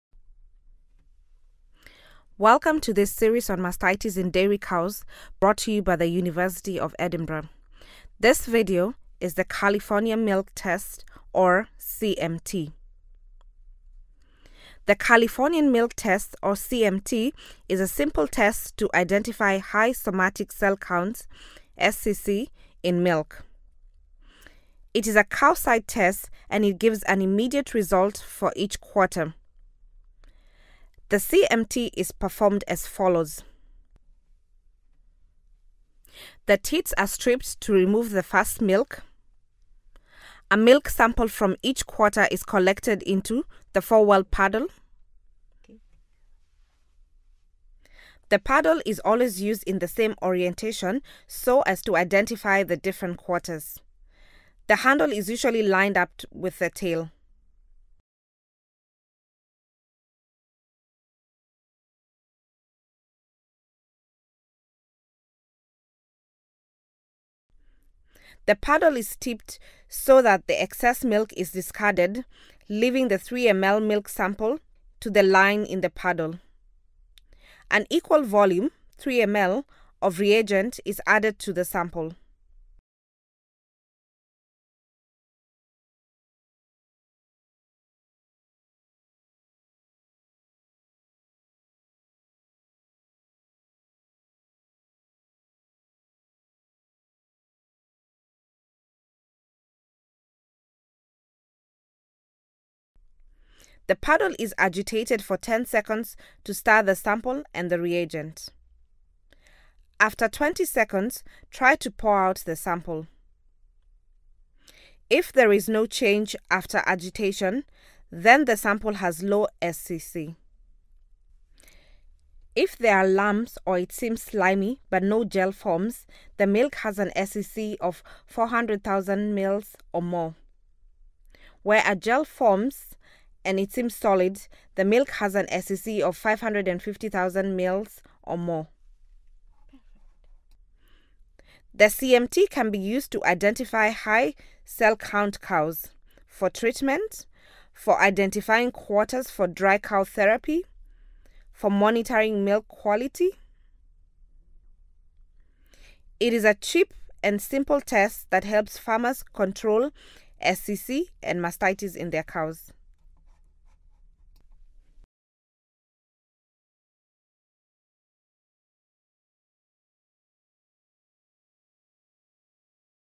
11: California milk test (CMT)
California milk test (CMT).

Supporting Evidence-Based Interventions (SEBI) mobilises and applies data and evidence to help the livestock community make better investments that improve livelihoods for smallholders in low and middle-income countries.